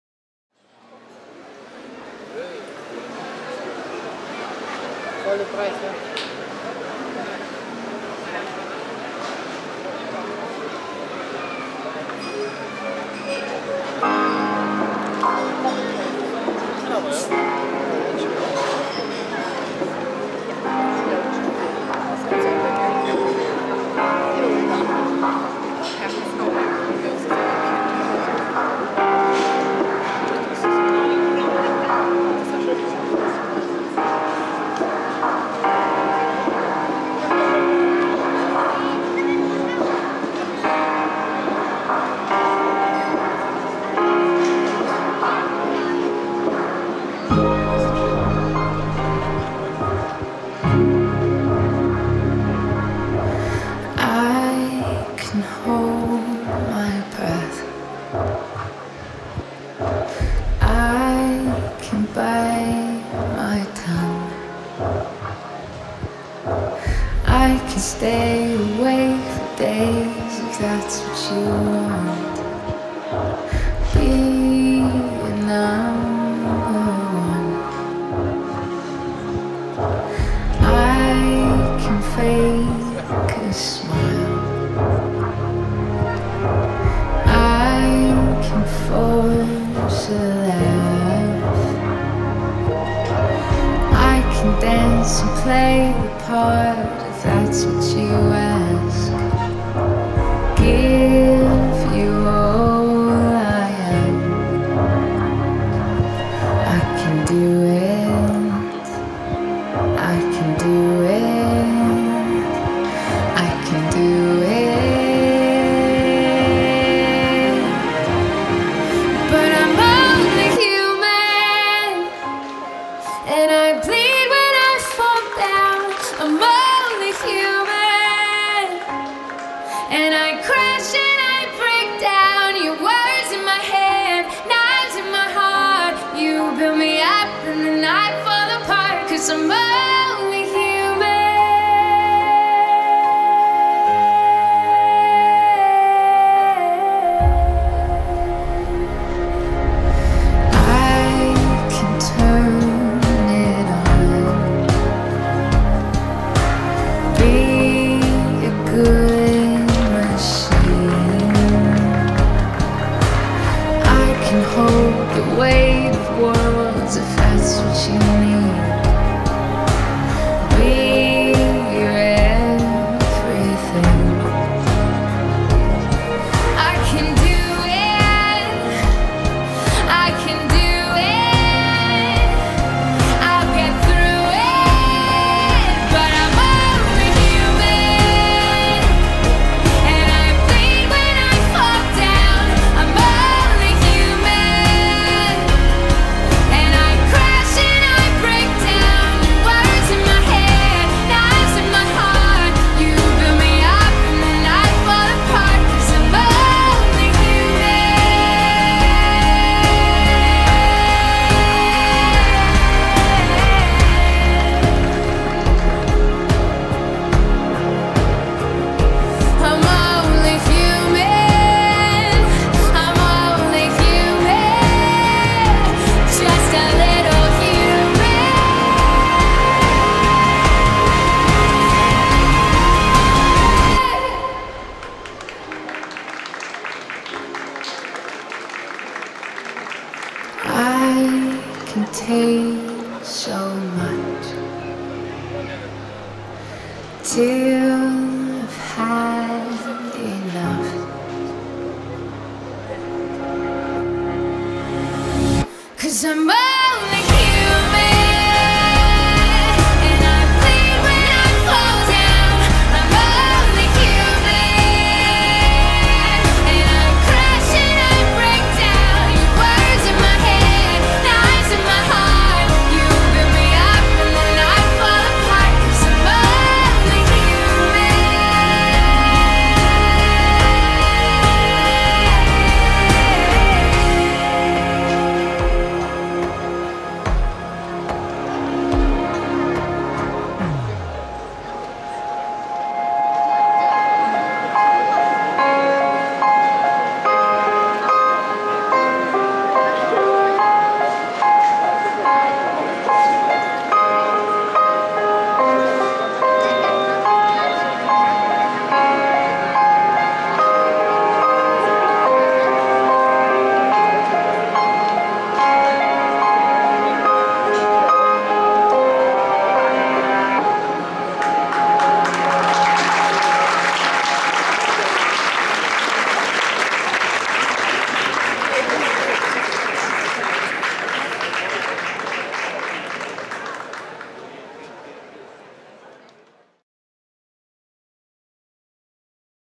Stuttgart - Street Performance - August 2015
Eine wunderschöne Street Performance mit einem sehr sympatischen Paar.
Aufgenommen am 22. August 2015 auf der Königstrasse in Stuttgart.

Gefilmt mit Sony HDR-CX690E
Kamera, Schnitt, Ton: PoMan/PoTecDE

Songwriters: Christina Judith Perri, Martin Johnston

Alle Rechte liegen bei den jeweiligen Eigentümern.